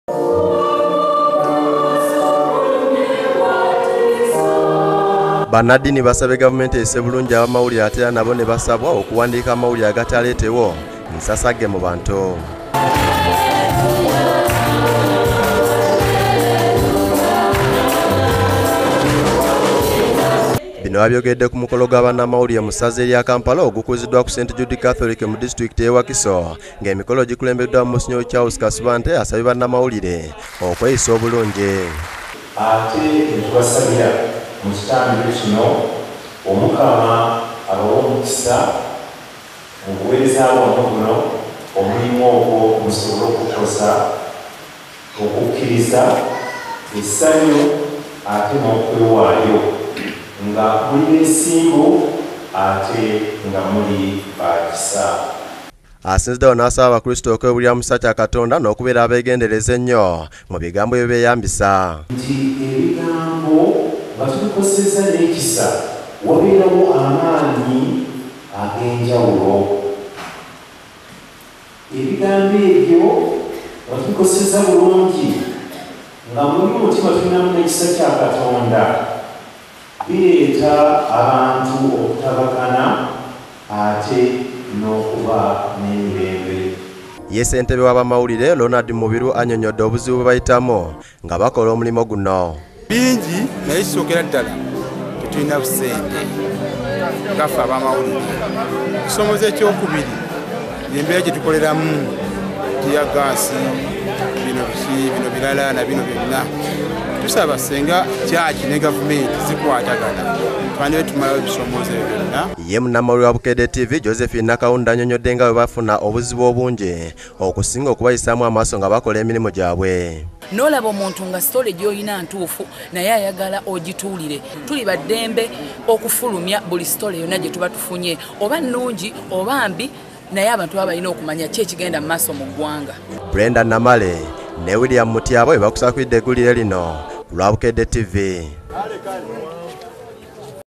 Abakristu bakuzizza olunaku lwa Bannamawulire
Basabiddwa okubeera abeesimbu mu mirimu gyabwe.